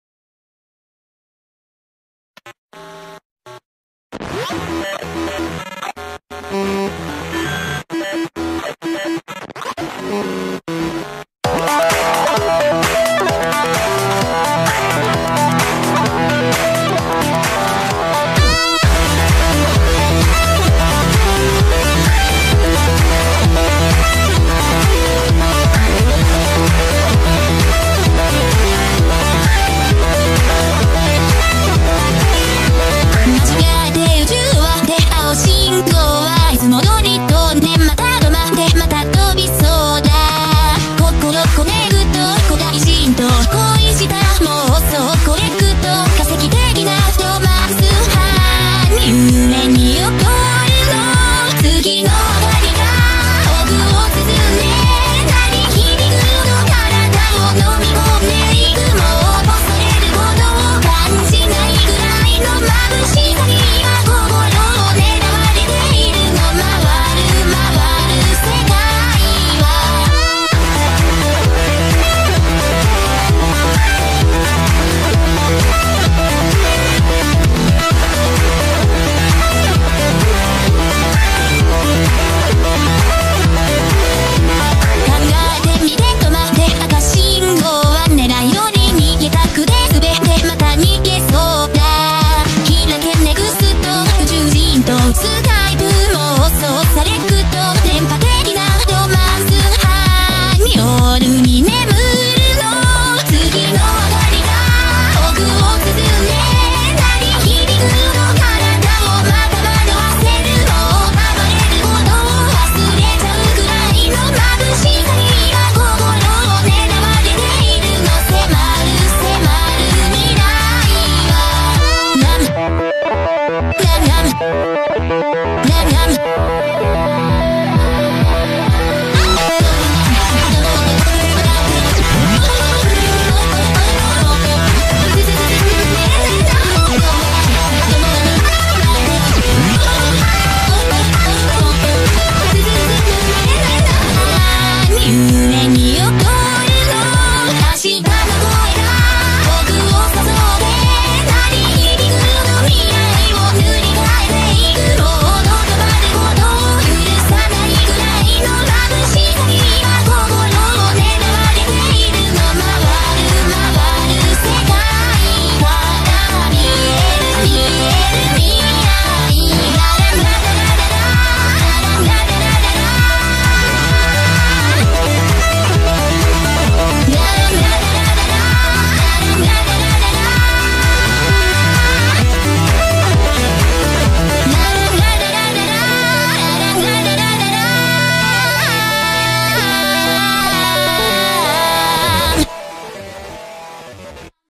[MMD] Model Test
This is just a smol test...I edited Gumi's clothing and hair textures a bit and gave her a different spa, but it's seriously hard to tell because of the effects...I also edited her hair a smol bit. And I made that Neru model. The only thing I don't like is how spazzy her hair is...Yeah ;3;